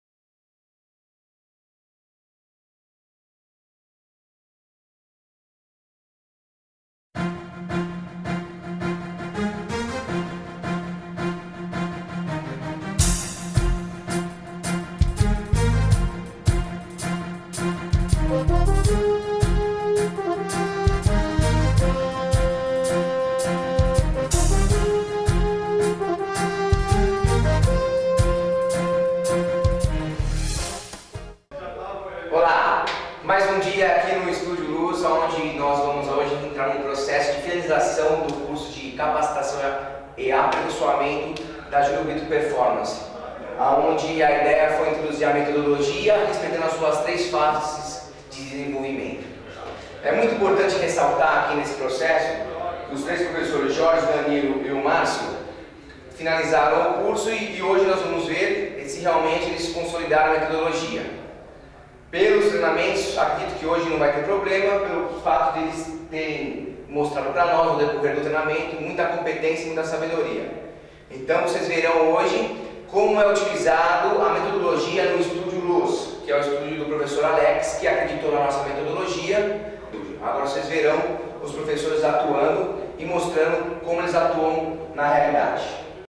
Treinamento Funcional/DVD - Studio Luos
Sinopse:

Este DVD mostra o Studio Luos, uma equipe de profissionais que concluíram os treinamentos propostos pela Julio Brito Performance. O Studio Luos apresenta uma série de exercícios comprovando que todos os seus profissionais estão preparados para aplicar a metodologia aprendida nos treinamentos que são propostos para seus alunos e atletas.

Duração
Aproximadamente 50min.

Acesse nosso site e veja mais DVDs de Treinamento Funcional.

Copie e cole o endereço abaixo e acesse!